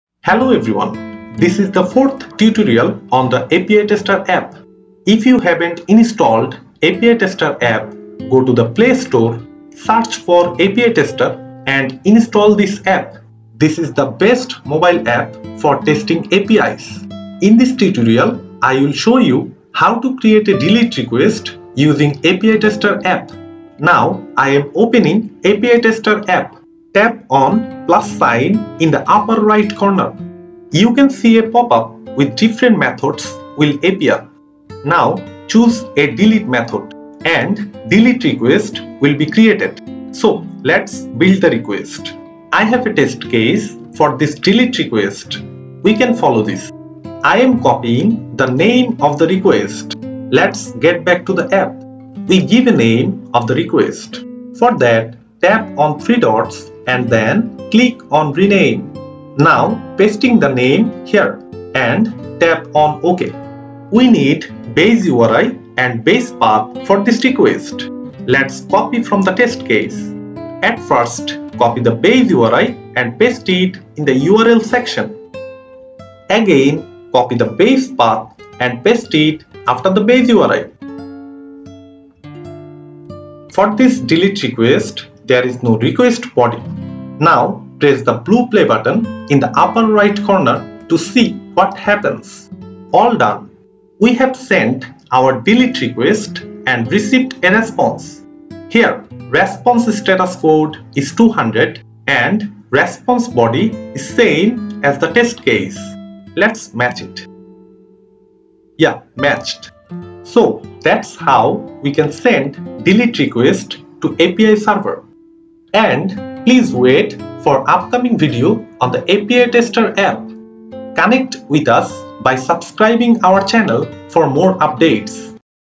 API Tester #4 Creating DELETE Request
Best mobile app to test any type of API.

This app is simple to use and has a user-friendly interface.
In this video, I showed how to send DELETE method easily using this app.